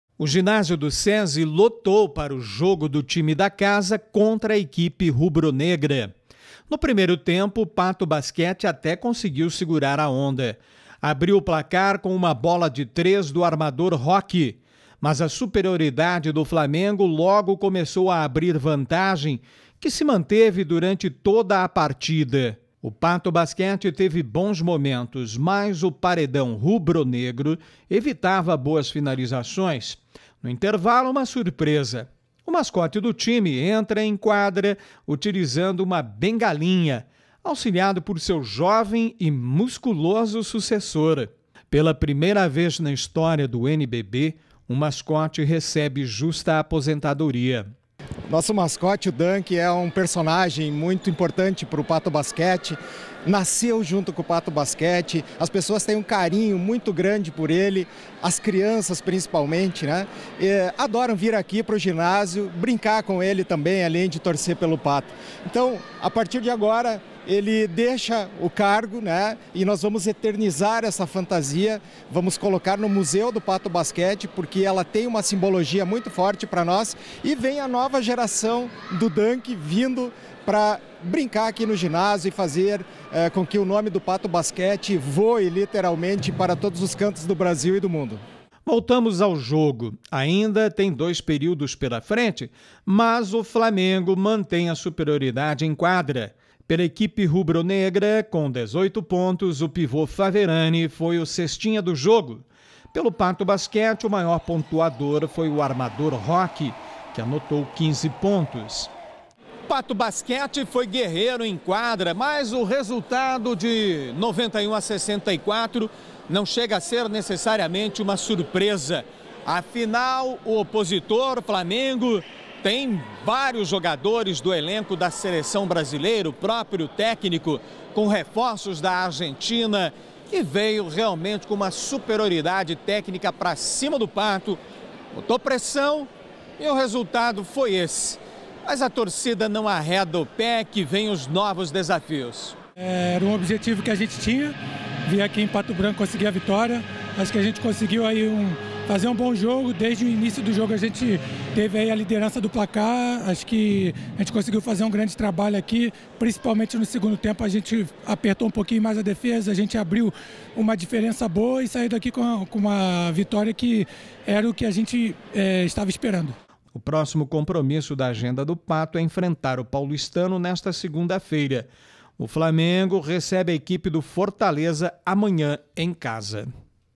Pato Basquete perde em jogo pelo NBB
Esporte: O Pato Basquete sofre derrota para o Flamengo.